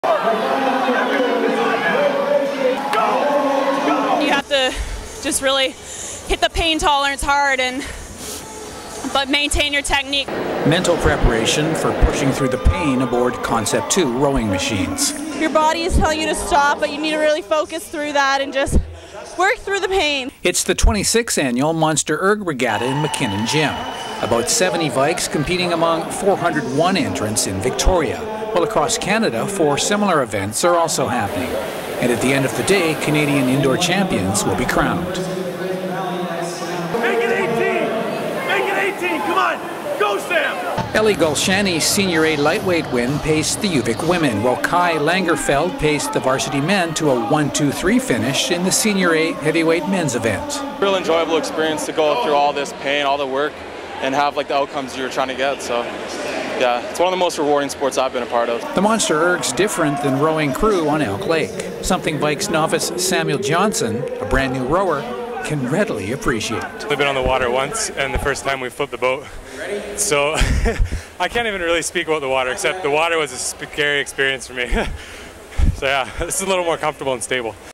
VIKES ROWING: Feb. 6 Monster Erg Recap
Vikes Videographer Norm LeBus gets footage and action from the Monster Erg held at McKinnon Gymnasium at the University of Victoria on Feb. 6. It's the 26th annual Monster Erg event, and is one of five events held across the country to crown the national women's and men's indoor rowing champion. on Feb. 7.